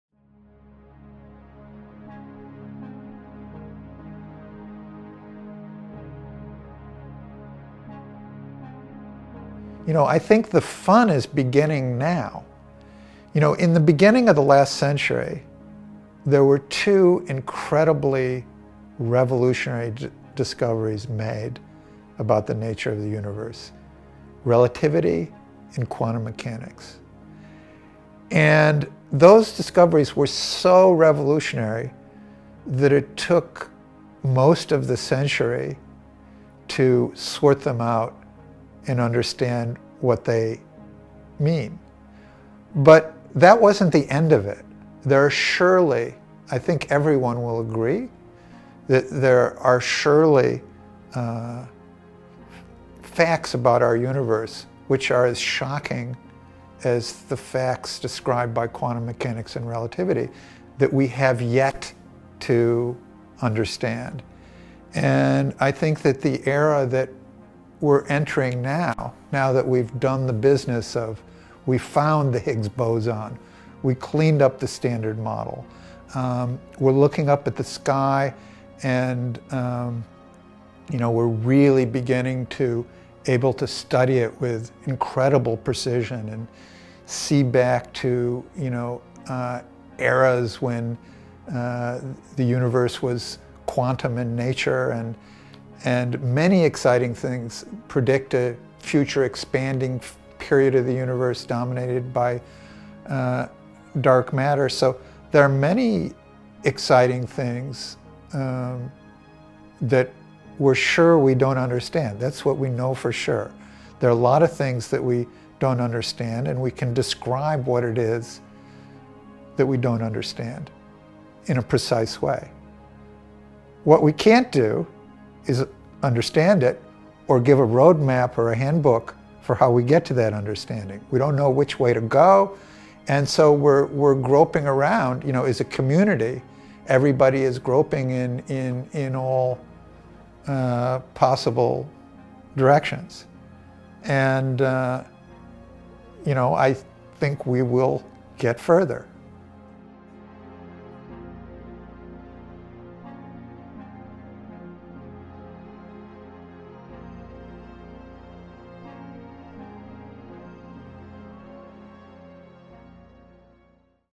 The Fun is Just Beginning | Andrew Strominger | The Search for the Theory of Everything
Physicist Andrew Strominger explains why the fun is just beginning in the search to understand the fundamental laws of nature.

Directed by: Elizabeth Puccini, Tristan Waldroop
Produced by: Elizabeth Puccini, Tristan Waldroop
Director of Photography: Tristan Waldroop
Edited by: Elizabeth Puccini, Tristan Waldroop